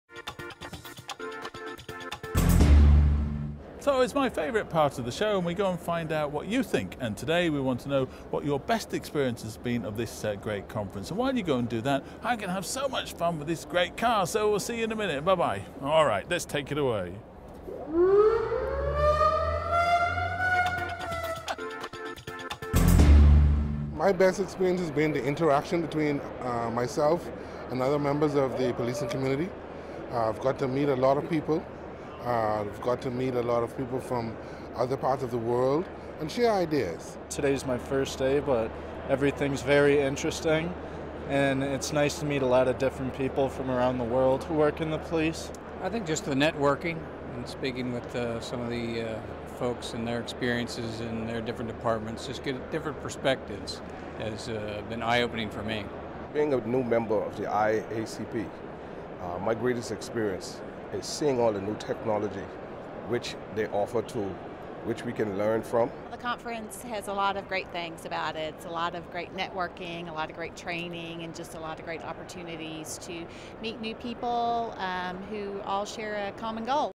Attendees Share Their Favorite Conference Experiences
Attendees share their favorite part of the IACP 2011 conference.